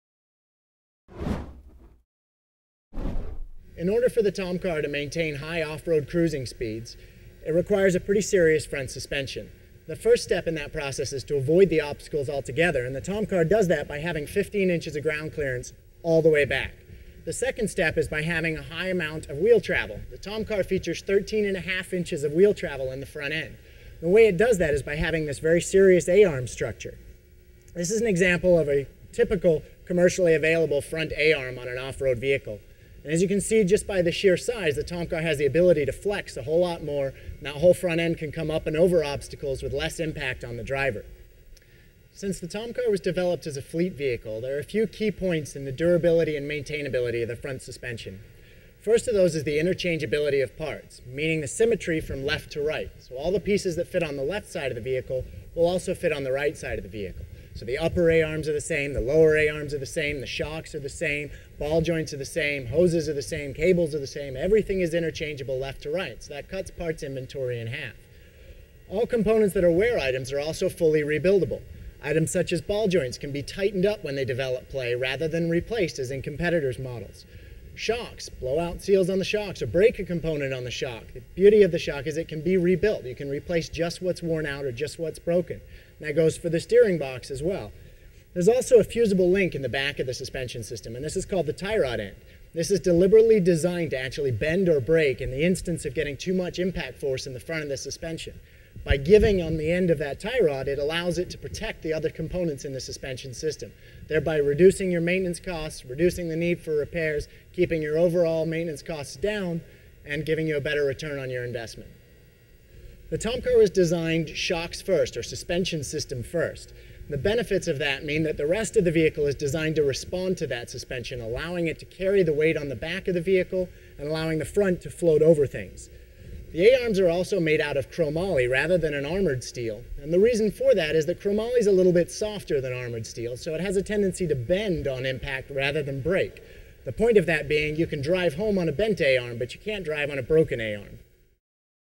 How It's Made - TOMCAR Front Suspension
Here's a quick look at Tomcar's front suspension system. Engineered to take you Anywhere and Back.